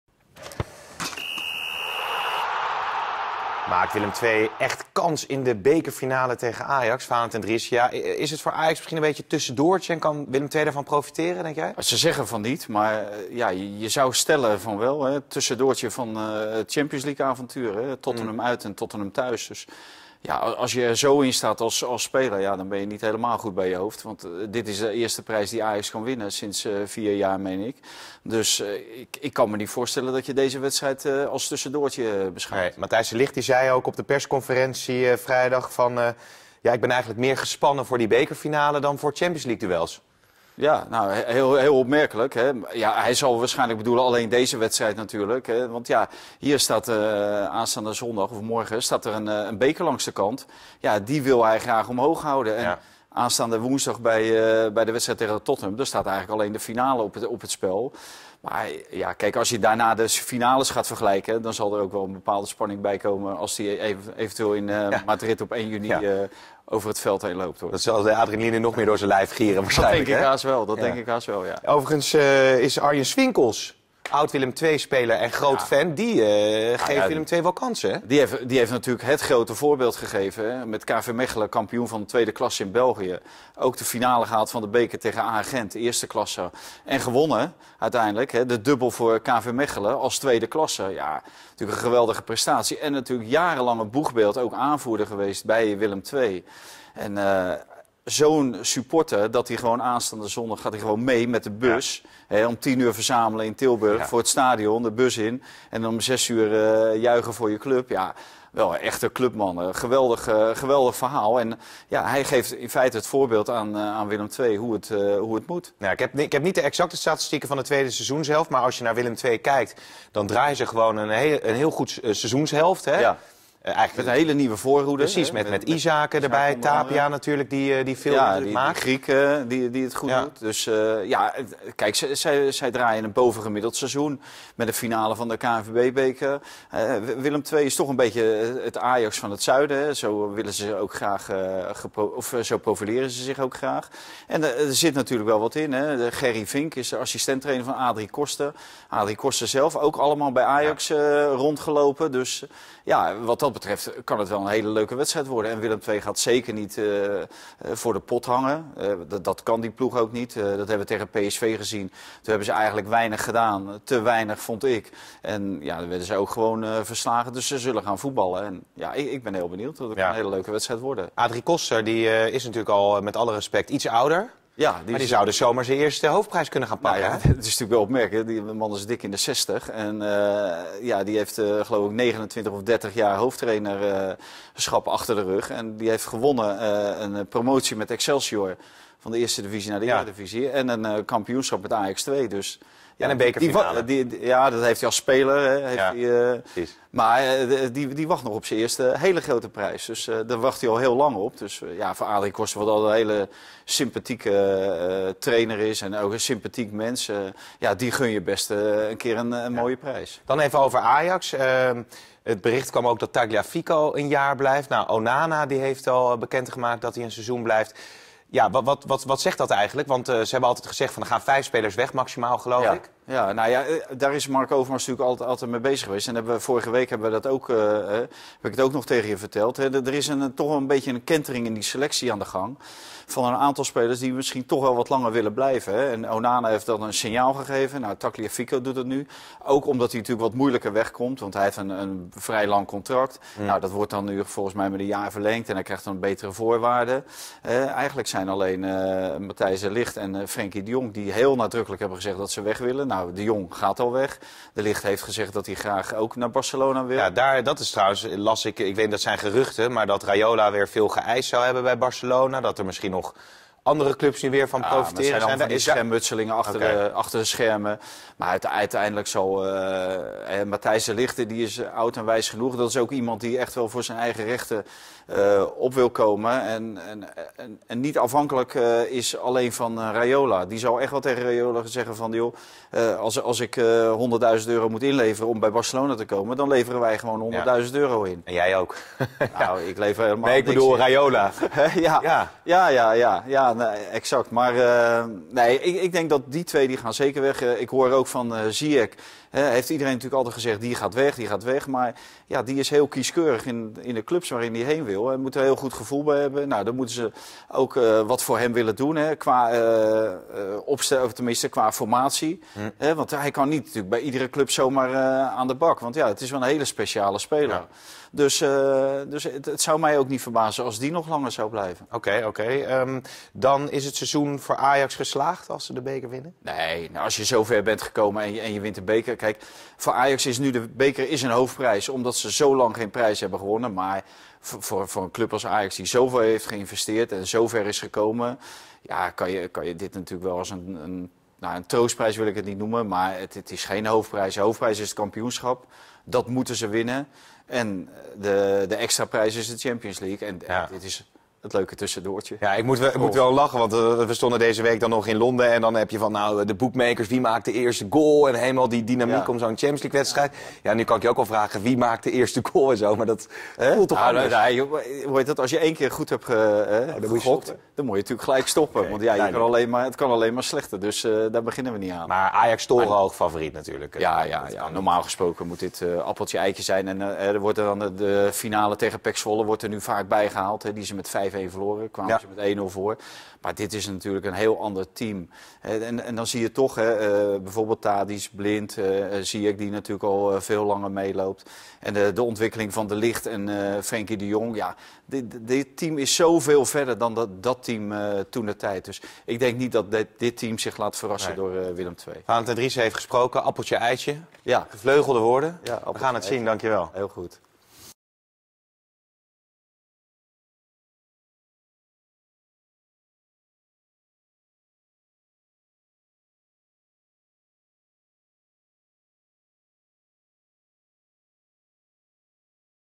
‘Kentering in selectie Ajax’
Pim Sedee blikt met chef voetbal Valentijn Driessen vooruit op de bekerfinale tussen Ajax en Willem II.